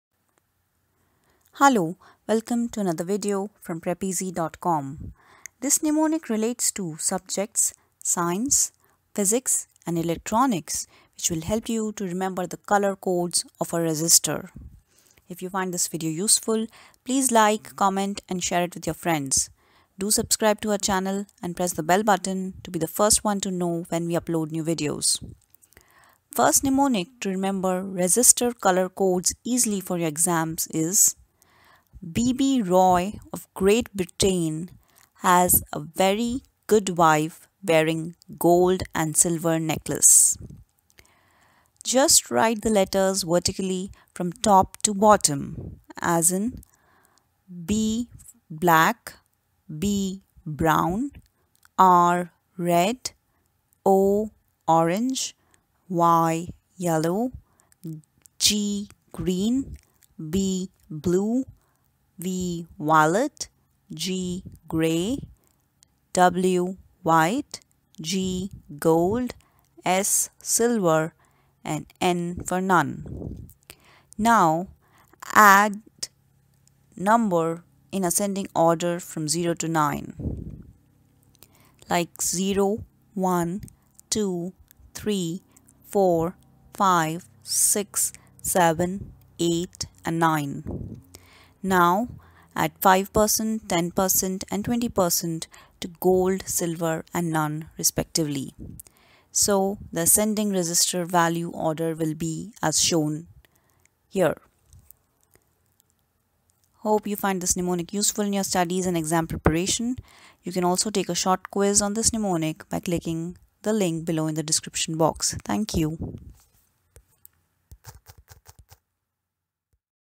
Trick to remember color codes of Resistor
This is a simple trick to remember the color codes of a resistor. This mnemonic will help you in many entrance exams like engineering entrance, medical entrance, PMT,IIT-JEE,etc.